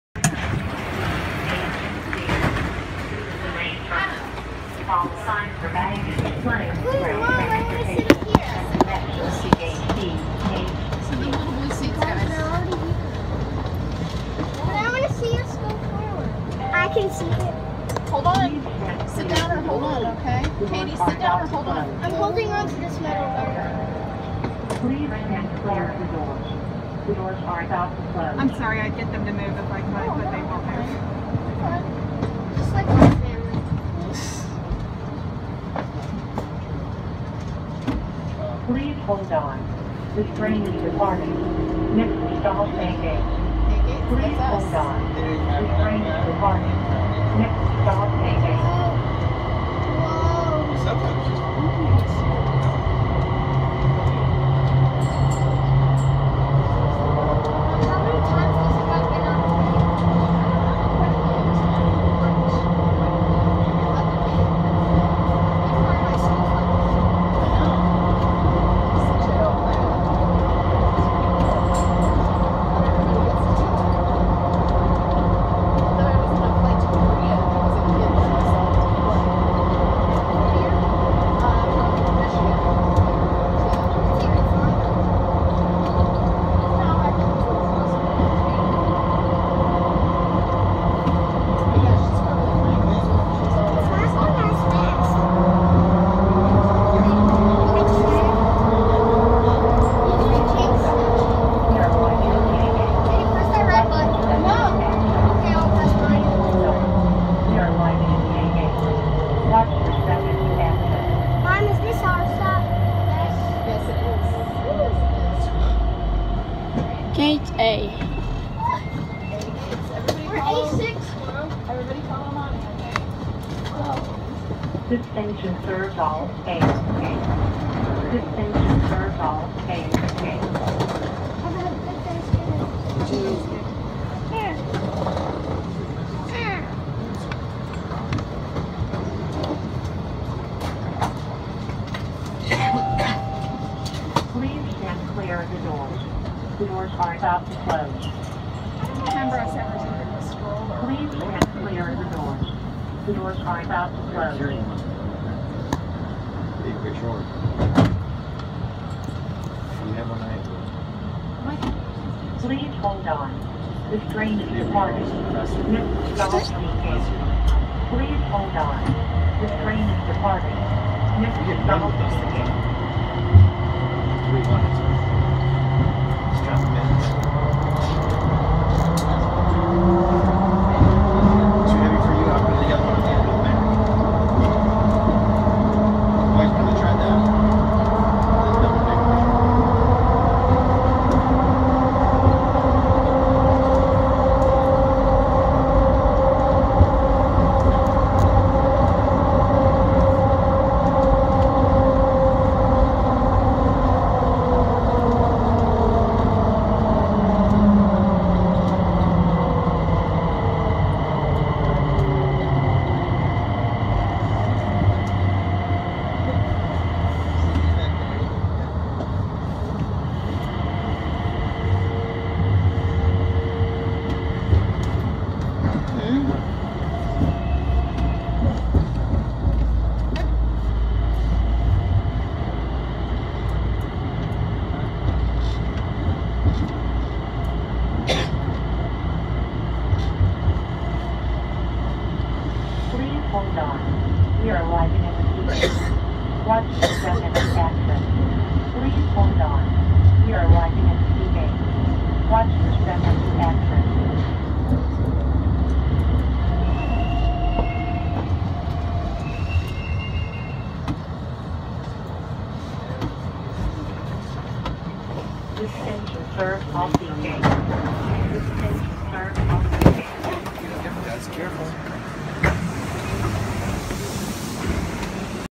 Washington Dulles International Airport AeroTrain Ride (From the terminal to C gates)
This is an underground train trip that takes us from the terminal to C gates in Dulles airport. Raw Footage Recorded in November 2015.